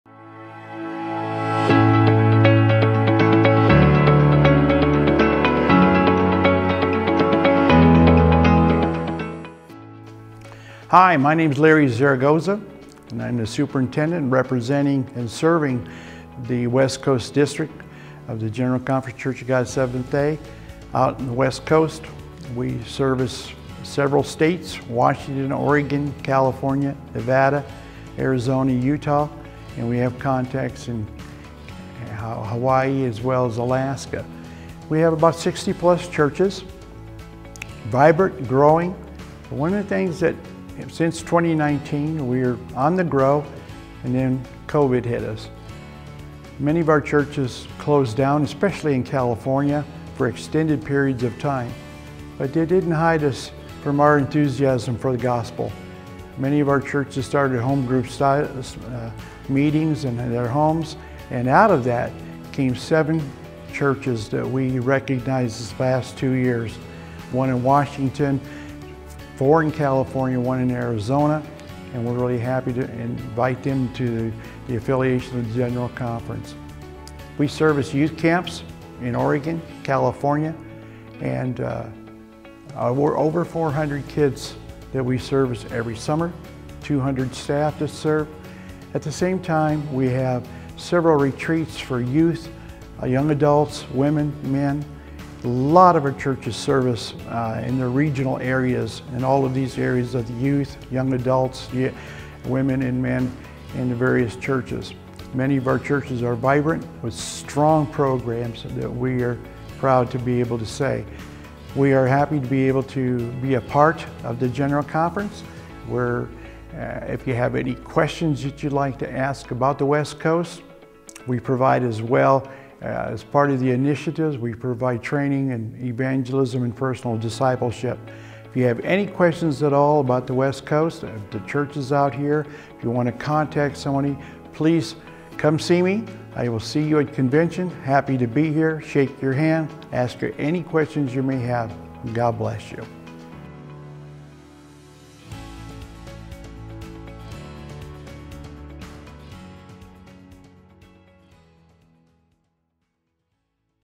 2023 BIENNIAL REPORT-WEST COAST DISTRICT
West Coast district superintendent, Larry Zaragoza, presented his biennial report of the work being done in his district.